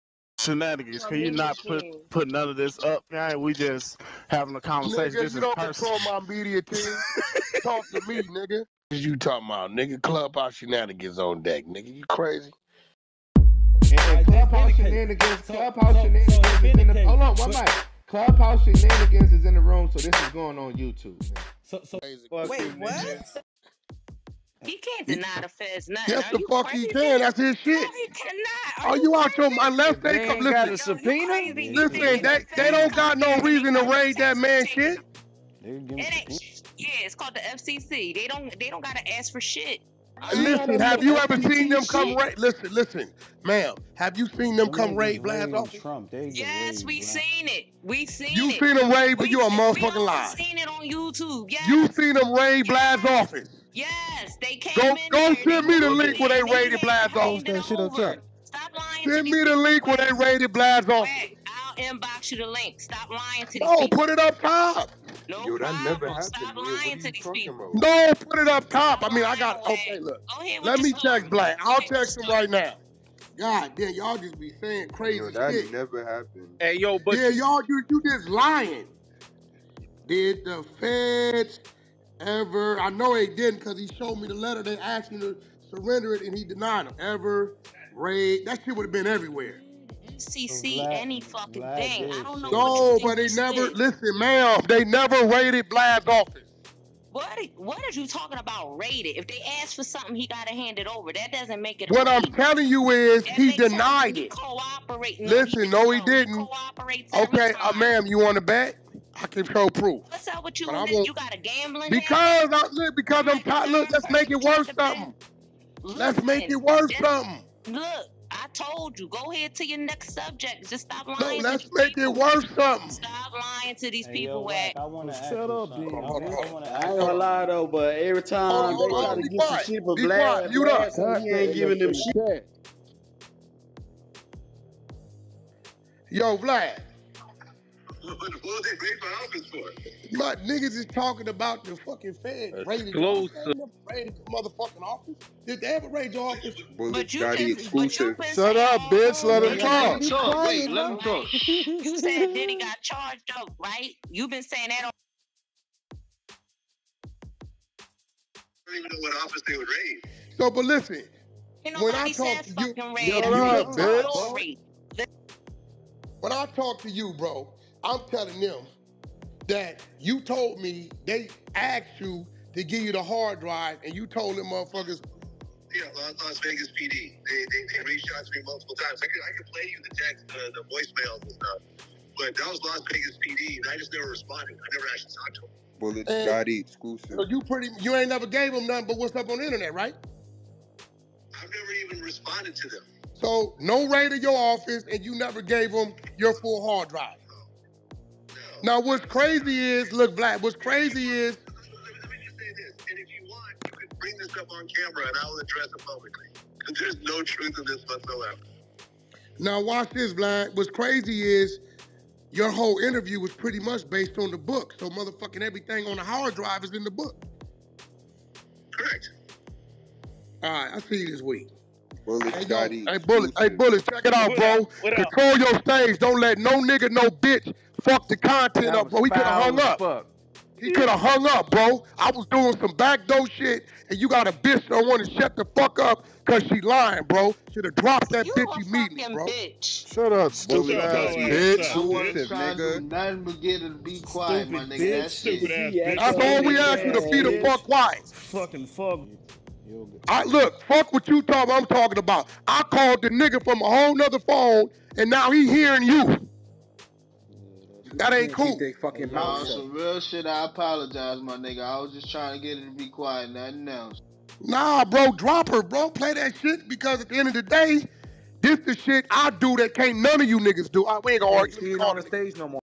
WACK 100 CALL DJ VLAD LIVE ON CLUBHOUSE ABOUT RUMORS
lilboosie

and go check out The 💯ENT and da trenches on Clubhouse and special thanks to @mickeytruth2022 @Clubh0useTv  @RaeGiveTheGoat

clubhouse1